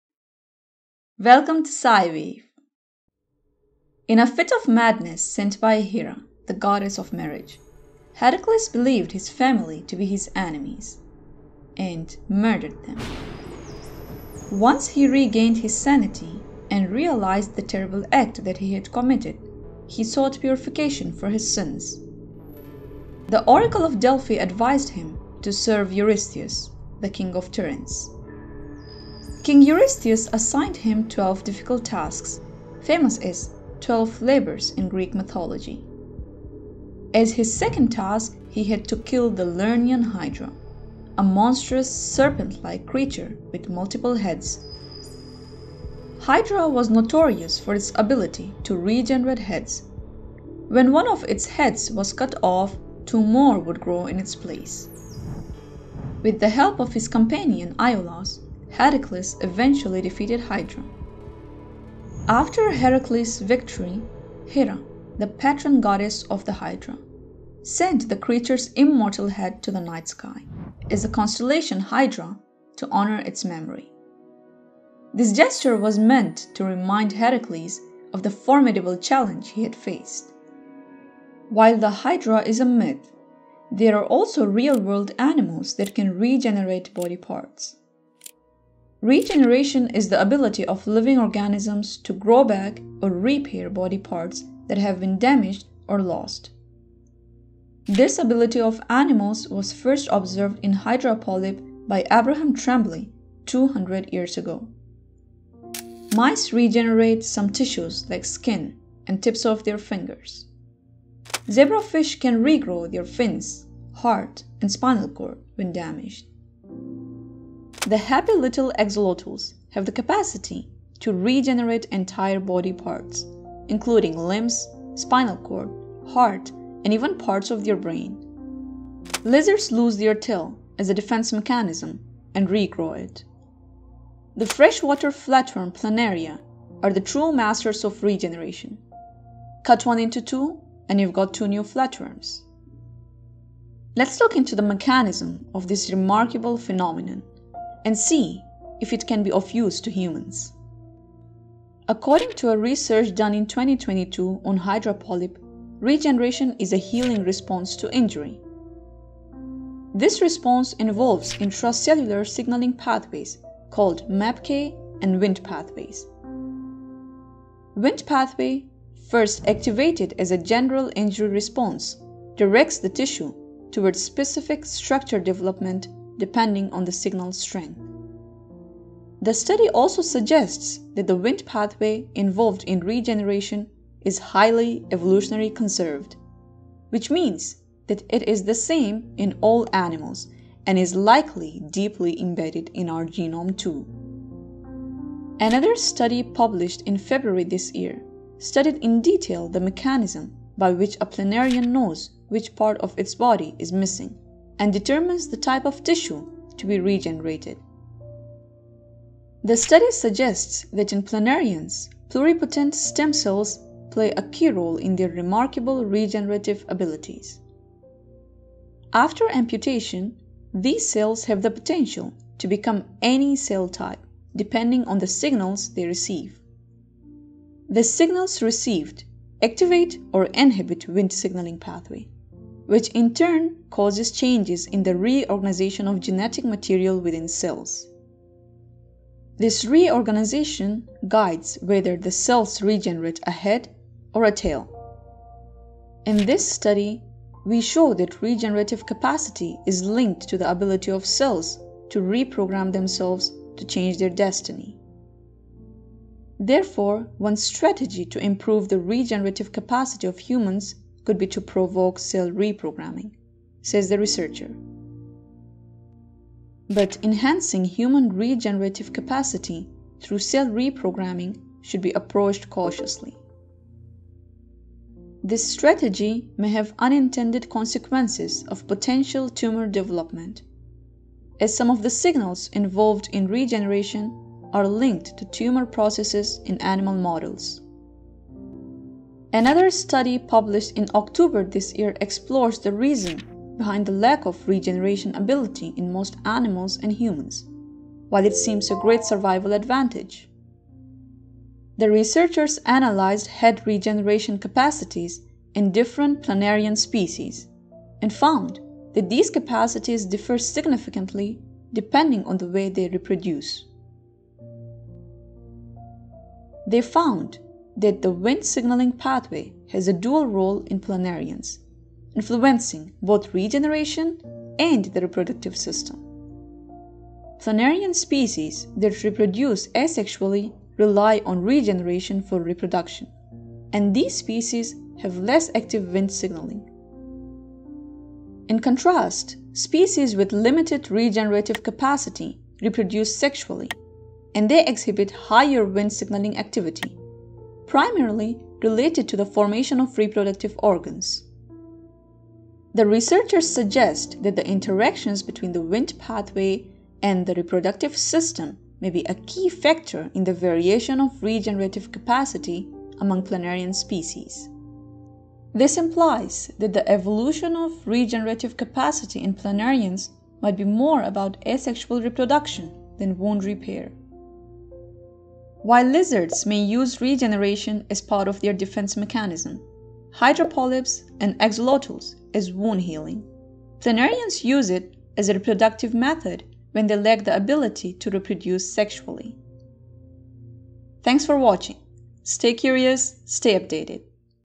Why Do Some Animals Regenerate Body Parts?
From the Hydra's mythical heads to the regenerative prowess of mice, zebrafish, axolotls, lizards, and Planaria, this video unravels nature's extraordinary healing powers, and intricacies of regeneration mechanisms—MAPK and Wnt pathways—delving into their evolutionary significance and potential implications for human regeneration.

Chapters:
00:00. Hydra's immortal tale
01:21. Regeneration introduction
02:30. Regeneration pathways
03:14. How is regeneration outcome determined?
04:05. Can human regenerative capacity be enhanced?
04:48. Regeneration's role in reproduction